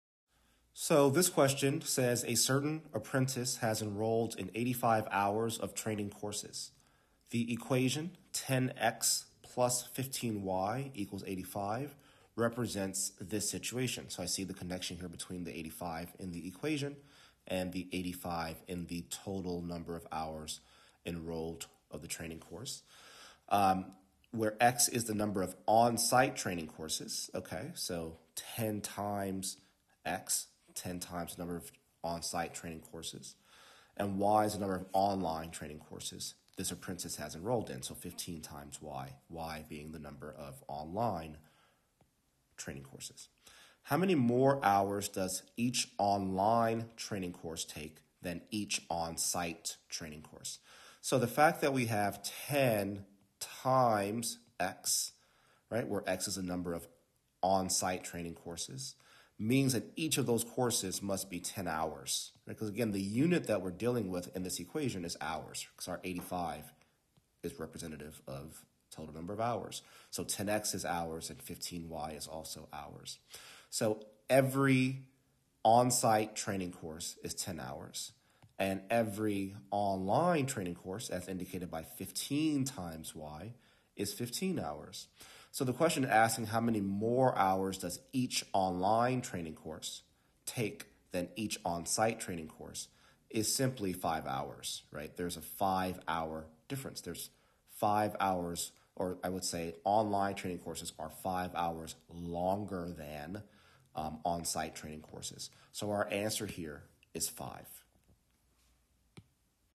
A certain apprentice has enrolled in 85 hours of training courses. The equation 10x + 15y = 85.....
Bluebook Digital SAT Test 4 Module 1 Question 6:

A certain apprentice has enrolled in 85 hours of training courses. The
equation 10x + 15y = 85 represents this situation, where a is the
number of on-site training courses and y is the number of online training courses this apprentice has enrolled in. How many more hours does each online training course take than each on-site training course?